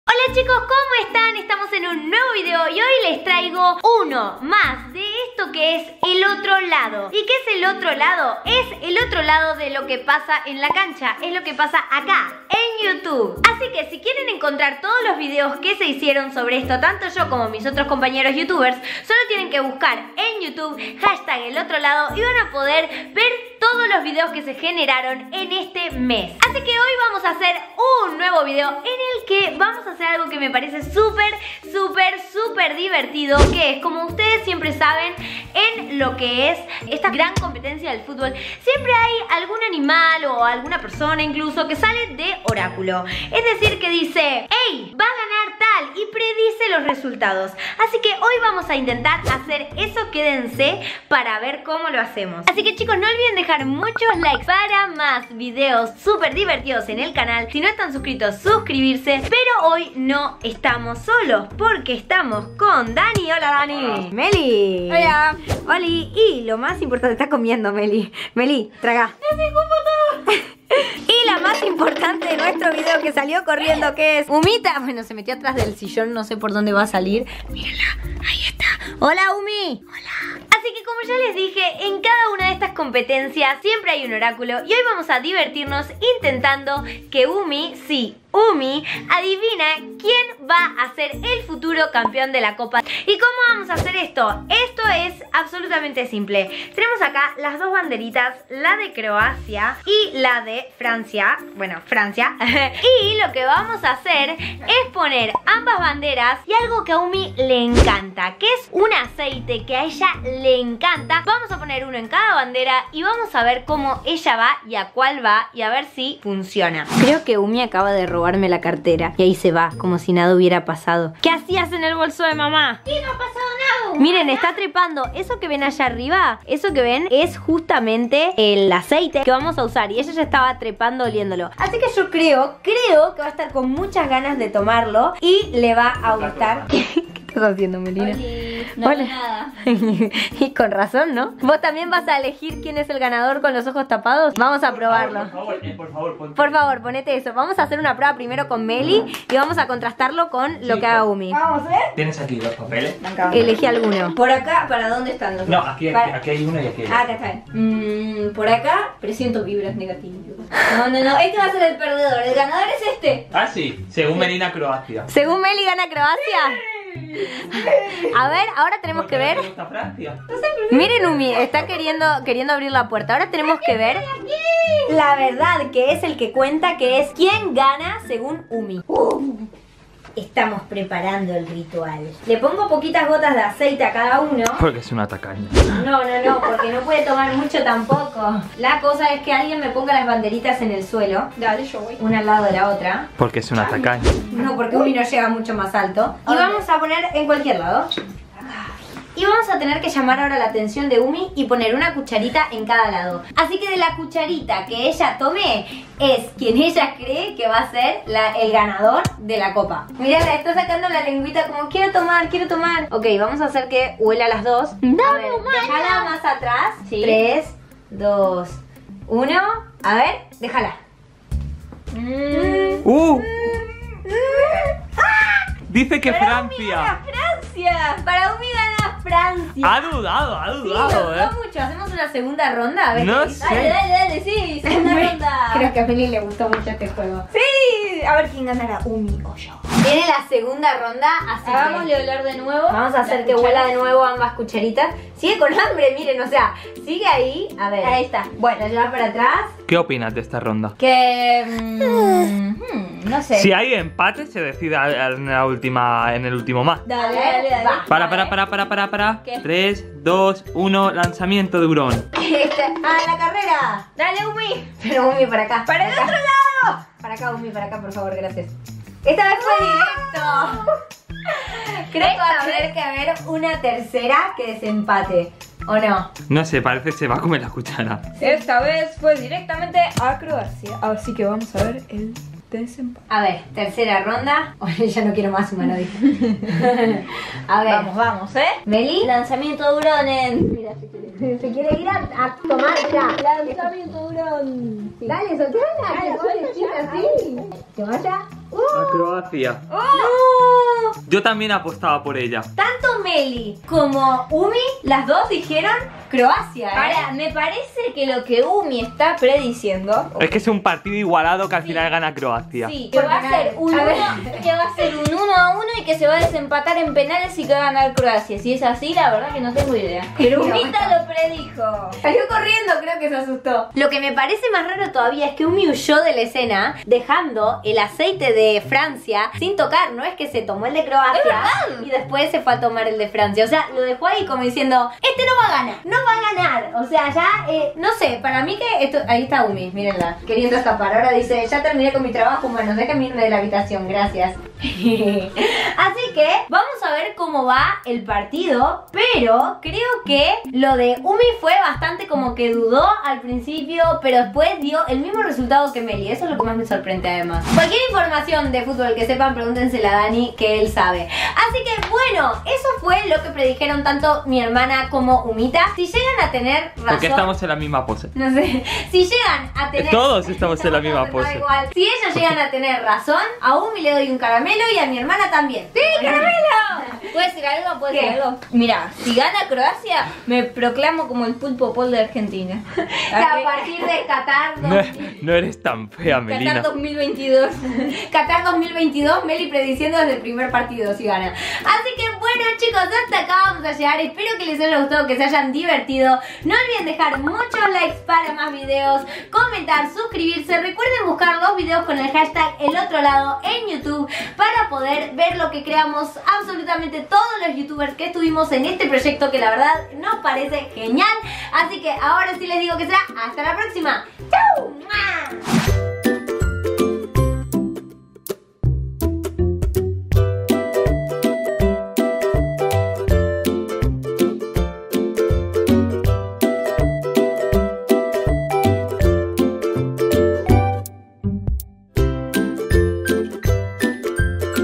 ElOtroLado | NUESTRO HURÓN PREDICE QUIÉN GANA LA COPA | Lyna Vallejos
En este nuevo video de Lyna Vallejos les muestro cómo nuestro hurón predice quién gana la copa en la final de Rusia por ElOtroLado! ♥ Like para más! ♥
↘️ ABRIR ↙️